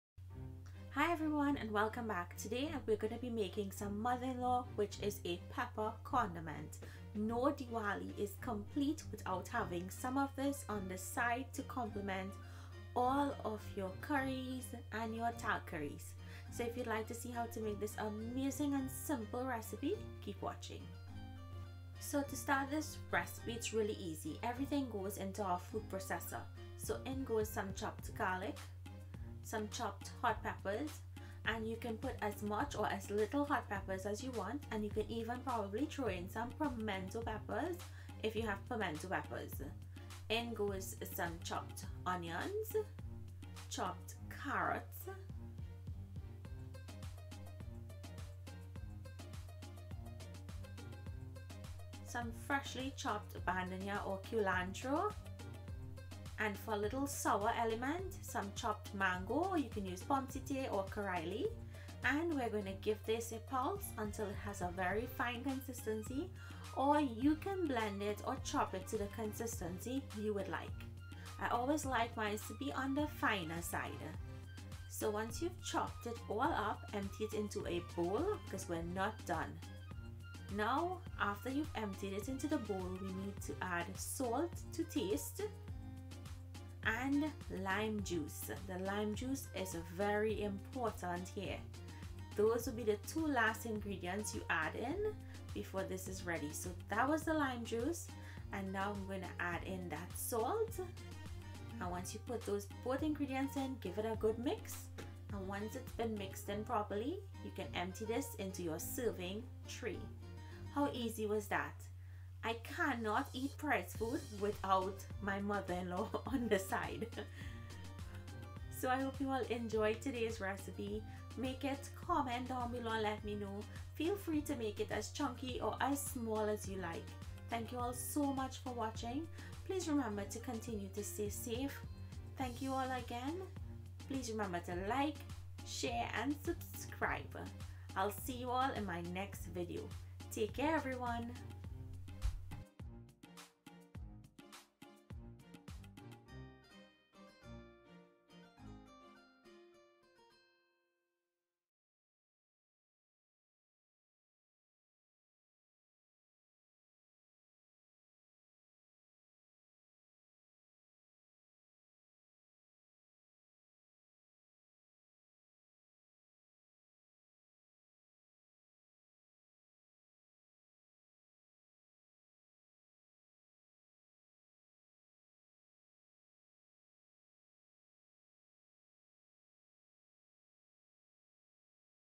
Trini Mother in Law / Mudda in Law / Pepper Condiment for Diwali - Episode 1115
4 scotch bonnet peppers
6 cloves garlic chopped
1/2 cup chopped carrot
1/4 cup chopped mango
1/2 small onion chopped
6 leaves bandania/ shadon beni /culantro
1/2 tsp salt
1 lime